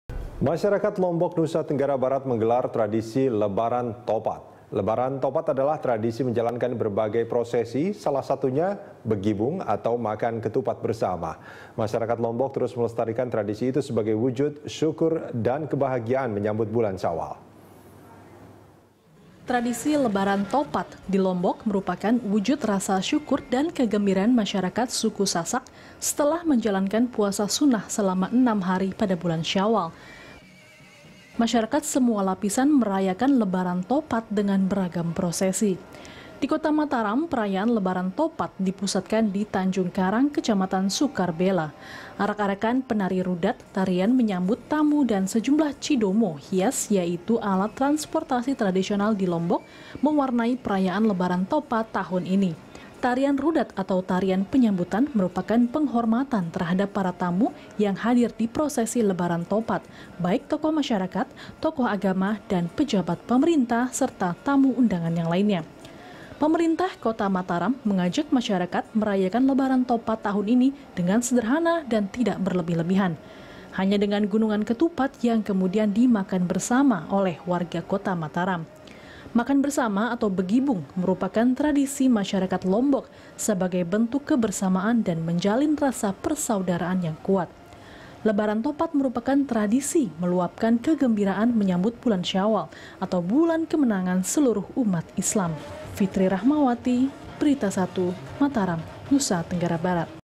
Masyarakat Lombok Gelar Tradisi Lebaran Topat
Masyarakat Lombok, Nusa Tenggara Barat, menggelar tradisi Lebaran topat. Lebaran topat adalah tradisi menjalankan berbagai prosesi salah satunya begibung atau makan ketupat bersama.

Facebook.com/BeritaSatuTV
Youtube.com/BeritaSatu